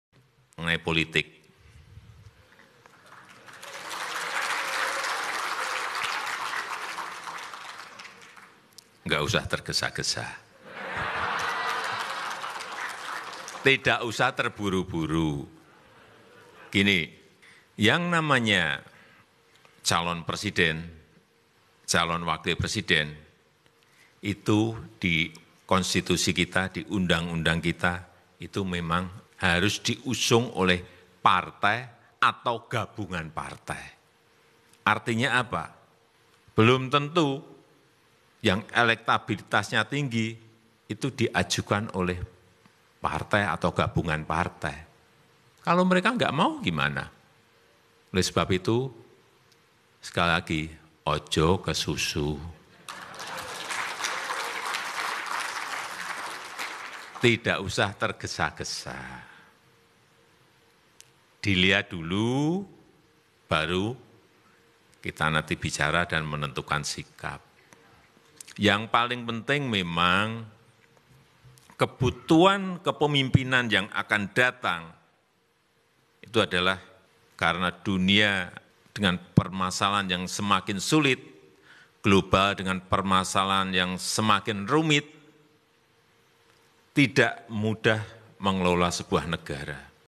Ini Kata Pak Jokowi tentang capres2024 pemilu2024 indonesia Tidak usah buru buru
Dari akun pak Jokowi